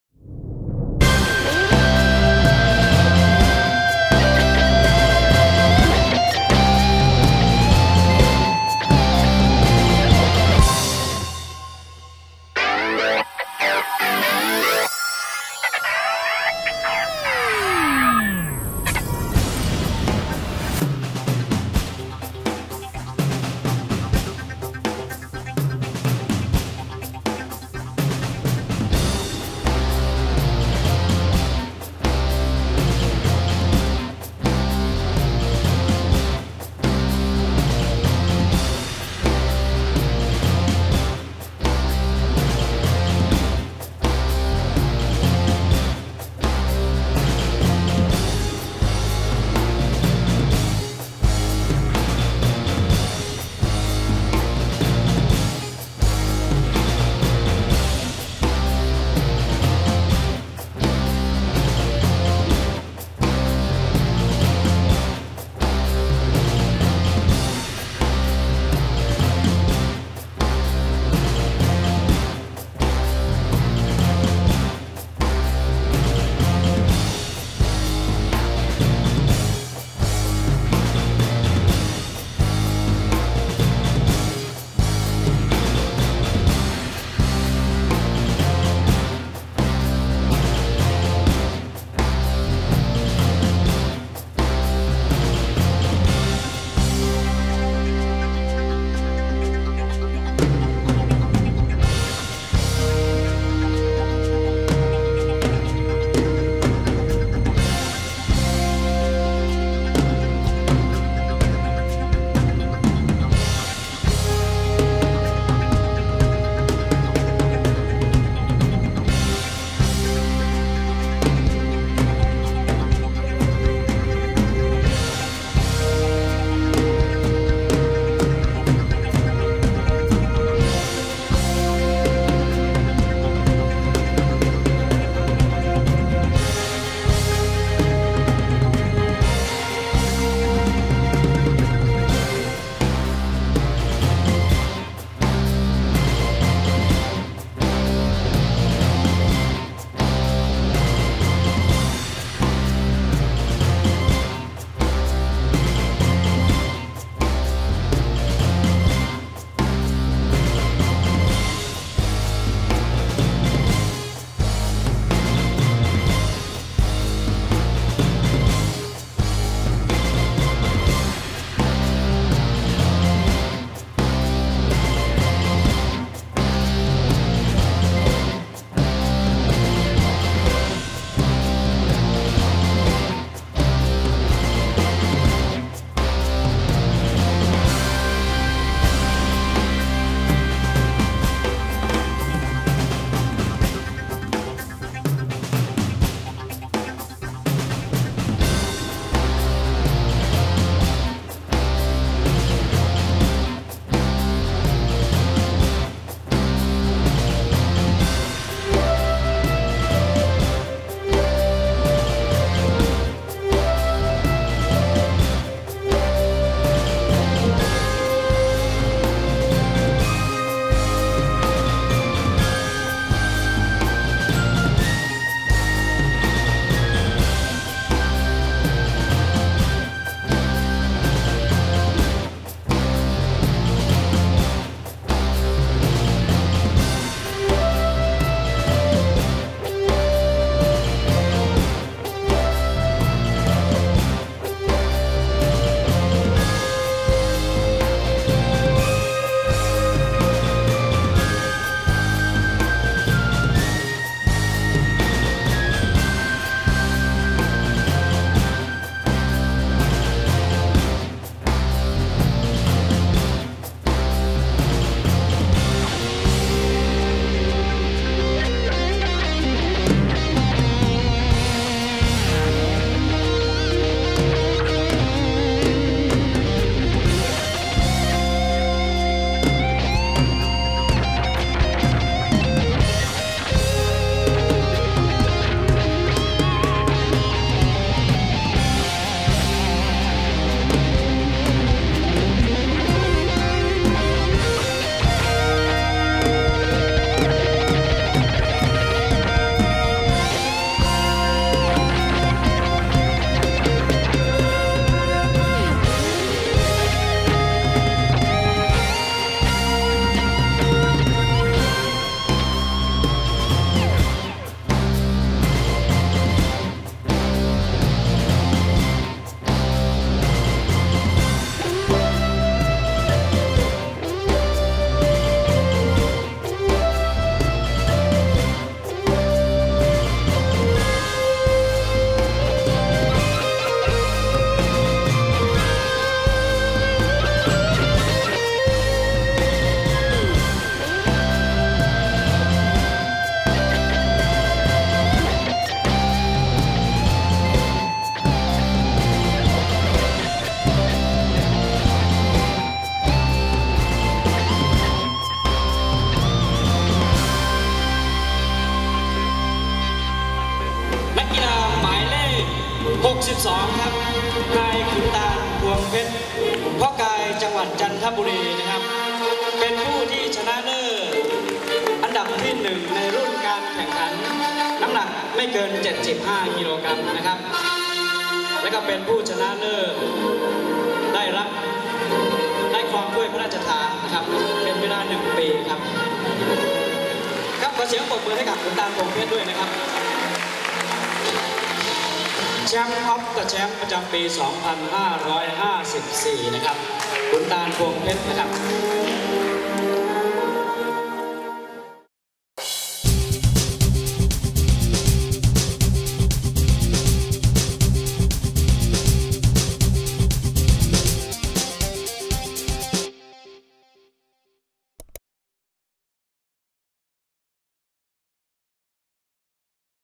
ThaiBody TV Podcast #100 - Mr. Thailand 2011 Preview
presents... Mr. Thailand 2011 took place at the Kwang Tong School in the Muang district of Sukhothai province.

Preview is featuring: Chakard Yenwattana; Tikamporn Surintorntha; Ron Nithiuthai; Arjharee Tansub; Wichit Singthong; Manop Yuin; Sakorn Mardwangseang; Samard Suansuk; Busabar Karkuen; Khanitha Makongsum; Nongyao Gosinam; Nathakorn Apsuwan; Wilyporn Wannaklang; Khuntan Puangphet; Wichien Jamjumlas; Seksan Sinseng; Chanchai Kongmoon; Wanchai Sangsuriyachat; Kuson Lasuer; Dhamrongsak Soysri; Jiraphan Pongkam; Wanchai Kanchanapimai; Worakorn Wongsakornmuang

ชื่อ : ฉกาจ เย็นวัฒนา; ฑิฆัมพร สุรินทรทะ; รอน นิธิอุทัย; อาจารี แท่นทรัพย์; วิชิต สิงห์ทอง; มานพ อยู่อินทร์; สาคร มาตรวังแสง; สามารถ สวนสุข; บุษบา ค้าขึ้น; ขนิษฐา มะคงสุข; นงเยาว์ โกสีนาม; ณธกร อาบสุวรรณ์; วิไลพร วรรณกลาง; ขุนตาล พวงเพชร; วิเชียร แช่มจำรัส; เศกสรรค์ สินเส็ง; ชาญชัย กองมูล; วันชัย แสงสุริยกฤษ์; กุศล ลาเสือ; ดำรงด์ศักดิ์ สร้อยศรี; จีรพันธ์ โป่งคำ; วันชัย กาญจนพิมาย; วรกร วงศกรเมือง

For the HD version, high quality photos, DVD's and many more bodybuilding and fitness video clips check out